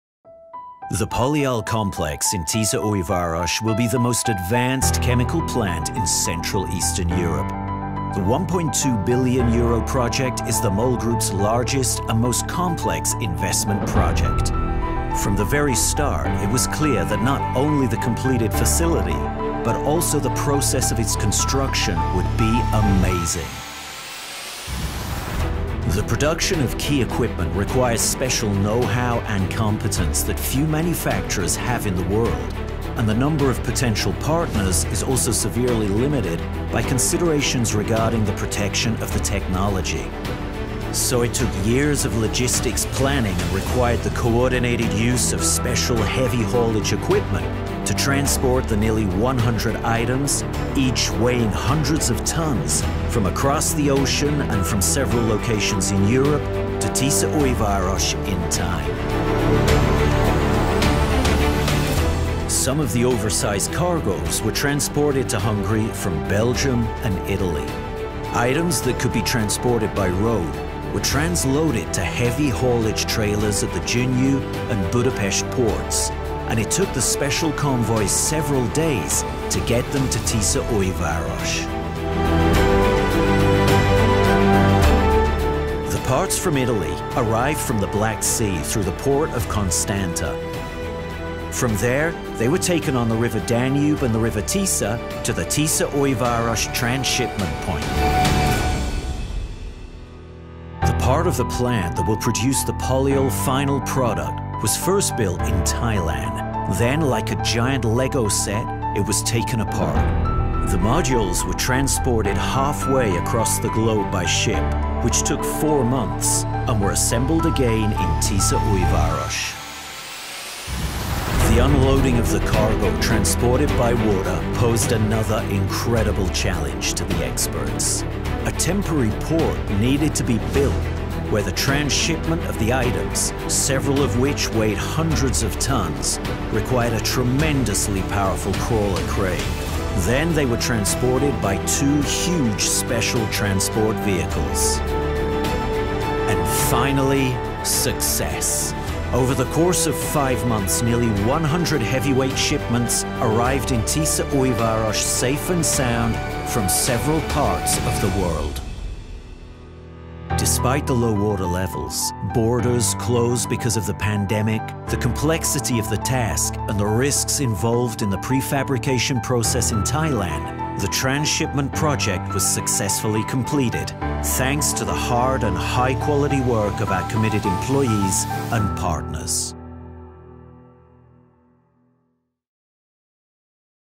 MOL's Polyol Complex in Tiszaújváros, Hungary
The video features MOL's Polyol Complex in Tiszaújváros, Hungary, the largest investment by the company in its downstream business and a noteworthy milestone to becoming a leading petrochemical company in the CEE region.
The video unveils the history of the project and its amazing construction complexities over a course of 5 months with nearly 100 giant shipments safely arriving at Tiszaújváros from around the world despite the obstacles due to the on-going pandemic.

Video Credit : MOL Group